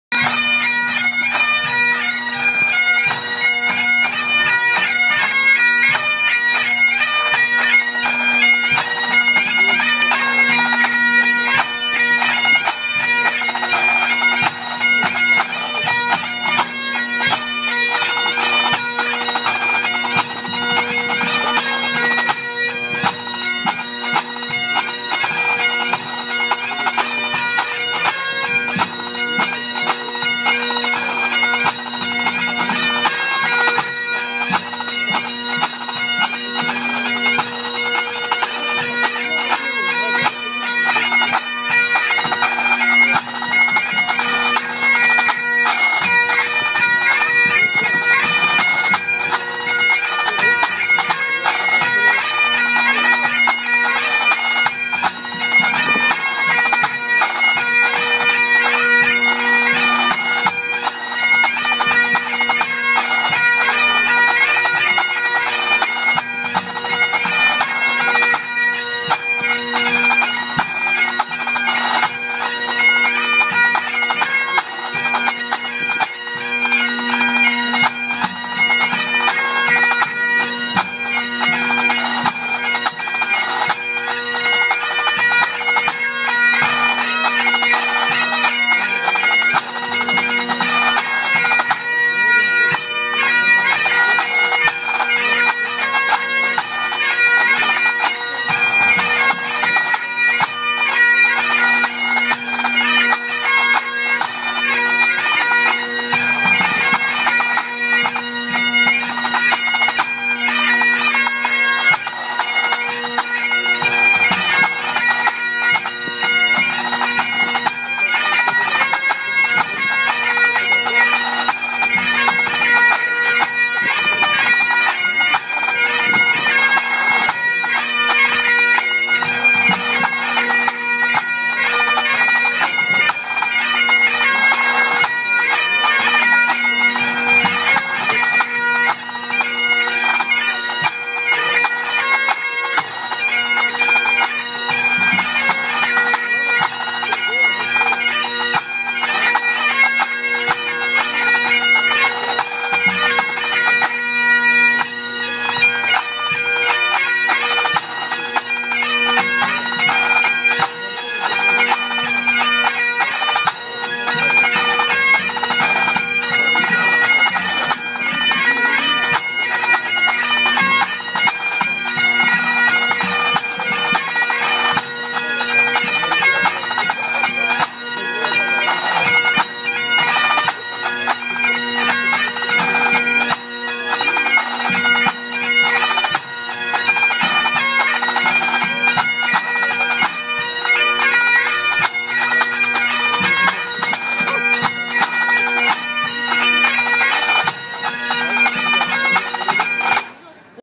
Cullen pipe band
In competition at Cobh 2nd June 2013